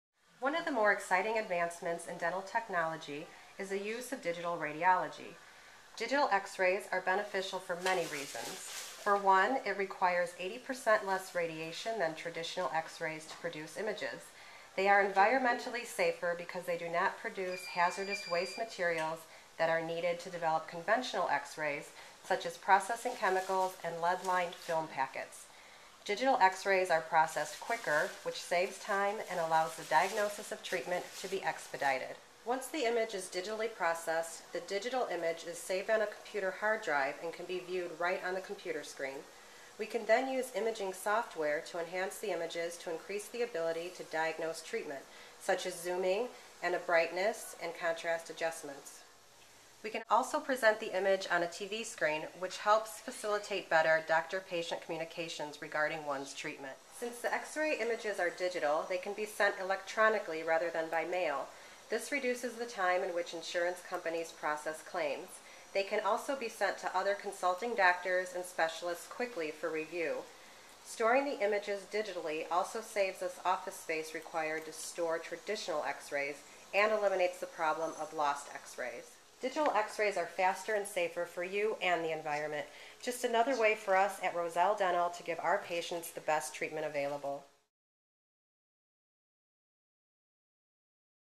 Roselle Dental Center-The Use of Digital Radiology-Digital X-Rays
The staff at Roselle Dental Center in Roselle, Illinois, discuss the benefits of digital radiology. Or, Digital X-Rays.